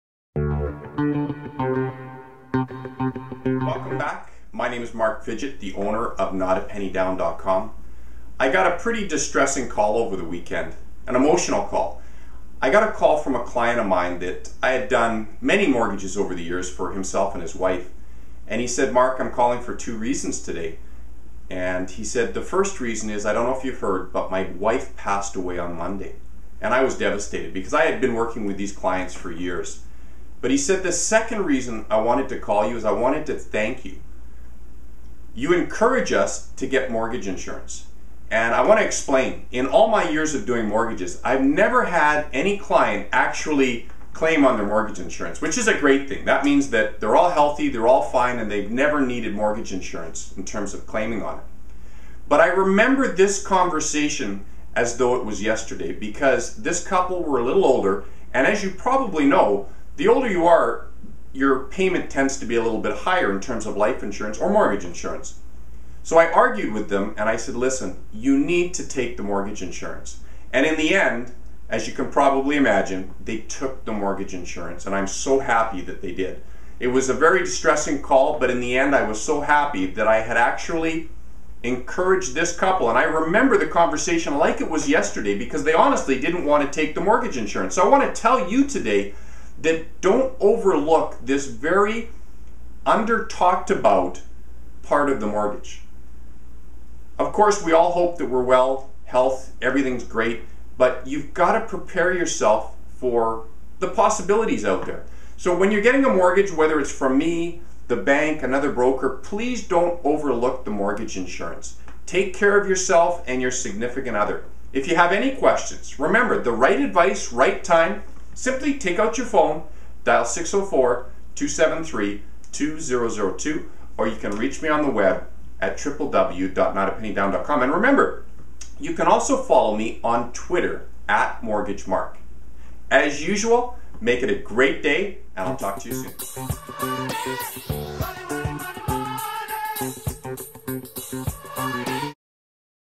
Mortgage Life Insurance with Vancouver BC Mortgage broker Mark Fidgett
. Mark Fidgett, a mortgage broker in Vancouver Canada, explains mortgages life insurance and how it affects the mortgage process.